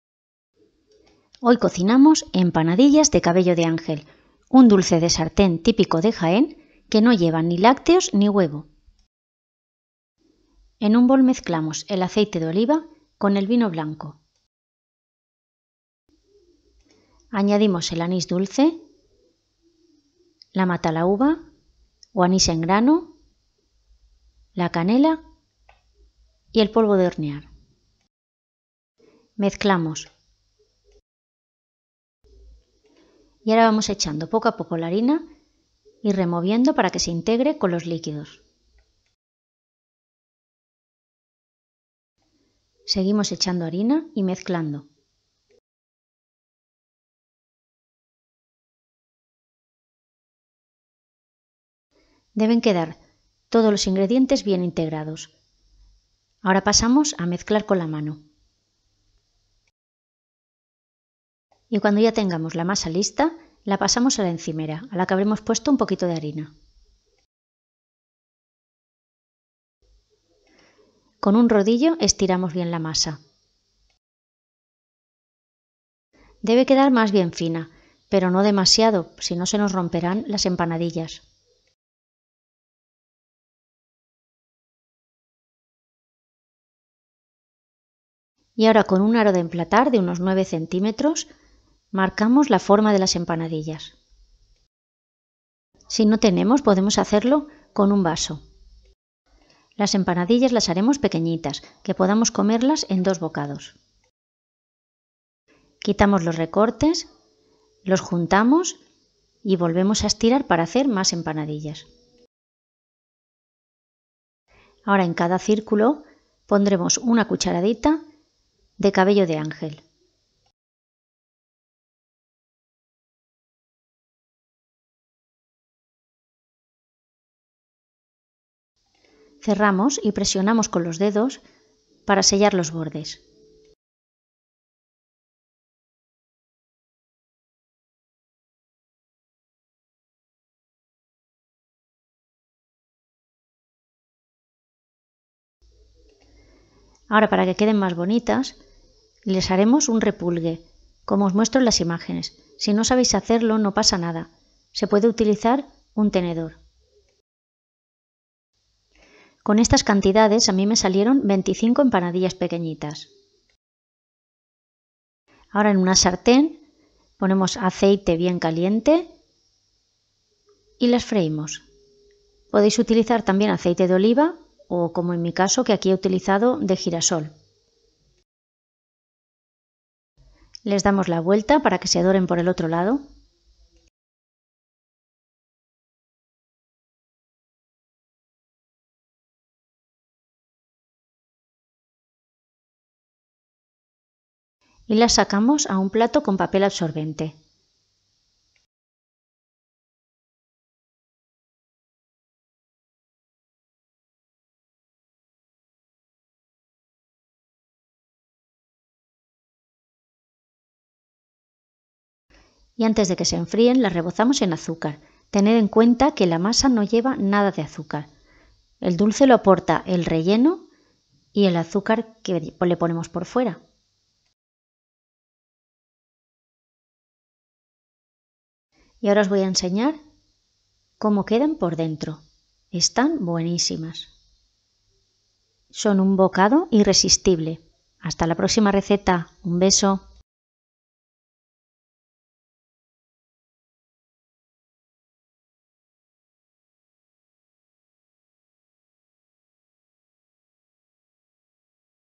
Empanadillas de cabello de ángel
Hoy prepararemos un dulce de sartén, unas empanadillas de cabello de ángel, típicas en la provincia de Jaén. Son unos dulces tradicionales riquísimos, que se preparan tanto en Semana Santa como en Navidad.

Ingredientes para 25 empandillas pequeñas:

100 ml de vino blanco
100 ml de aceite de oliva vírgen extra
1 cucharada sopera de anís dulce
1 cucharadita de matalahuva o anís verde
1 cucharadita de canela molida
300 g de harina normal
1 cucharadita de levadura para bizcochos
1/2 lata de cabello de ángel
aceite de oliva o de girasol para freír

¡Gracias!

azúcar para rebozar